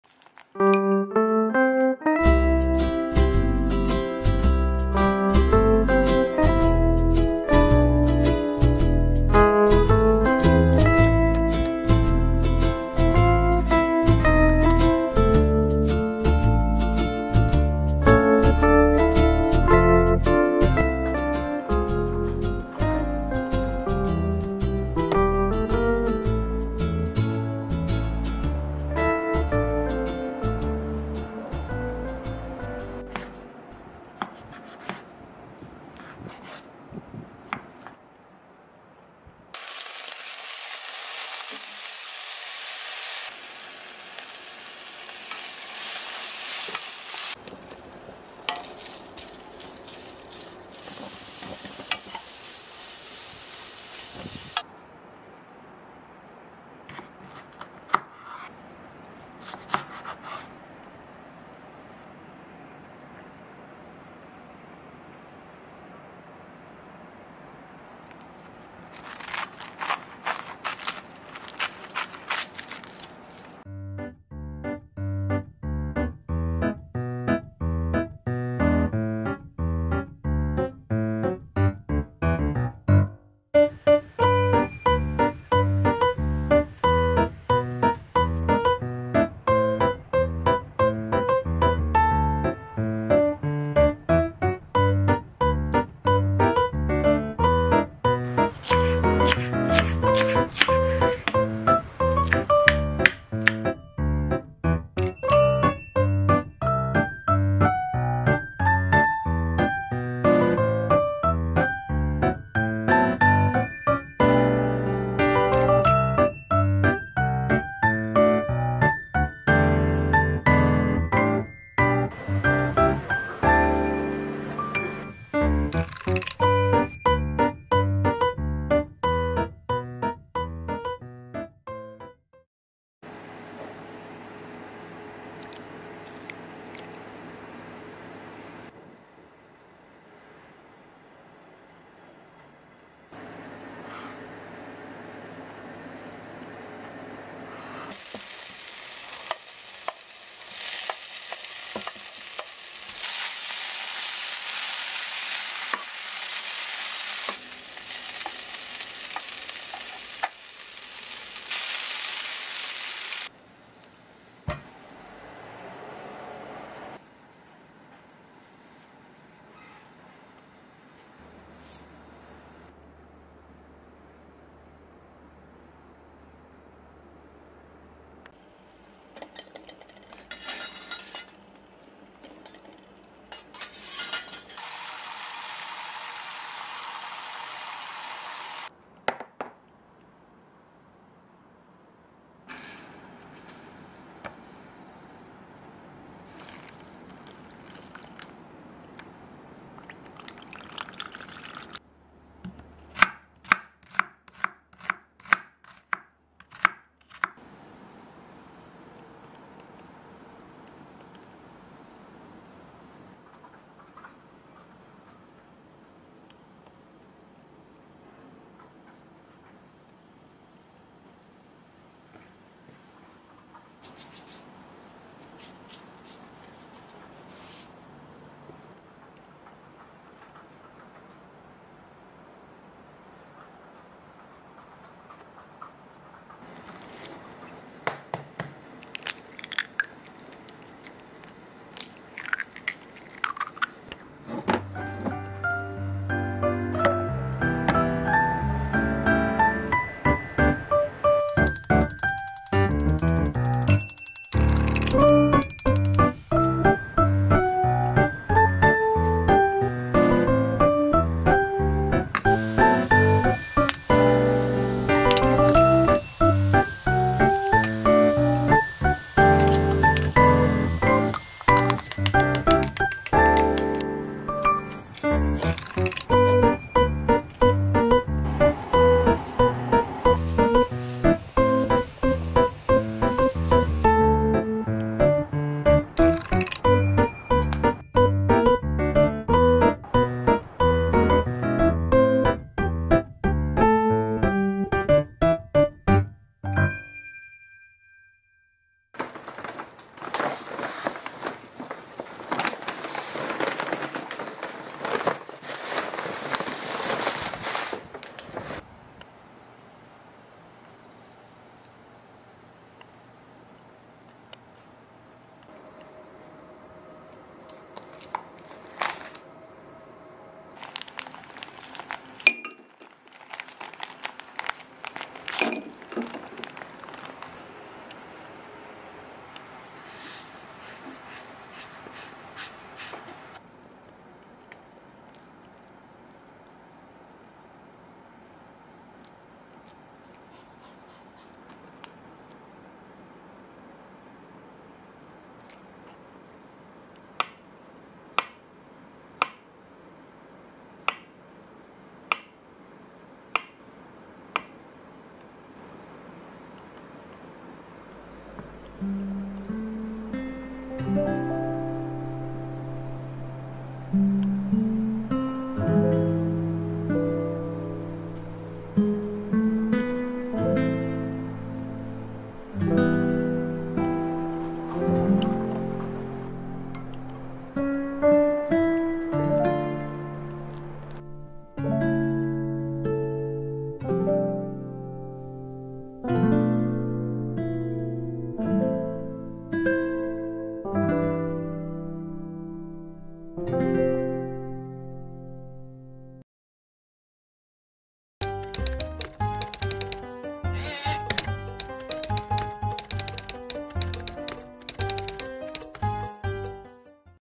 이웃집라이프 집밥브이로그 | 밑반찬 만들고 알뜰하게 식재료 파먹은 일상 (ft. 꼬마김밥, 해물라면, 감자채볶음, 계란장, 비빔당면, 아보카도명란밥)
_영상 품질을 1080p로 설정하시면
깨끗하면 선명한 화질로 보실 수 있어요 :D

안녕하세요 :)
여기는 이웃집, 저는 이웃집 사는 여자입니다 :)
김밥 한 번 해 먹고 남은 재료들 싹싹 긁어 요리하고,
궁금했던 냉동아보카도로 요리 해 먹은 일상이예요.

구독.좋아요 누르시고 저의 이웃이 되어주세요😍
애정어린 댓글로 함께 해주셔서 감사합니다🙏🏻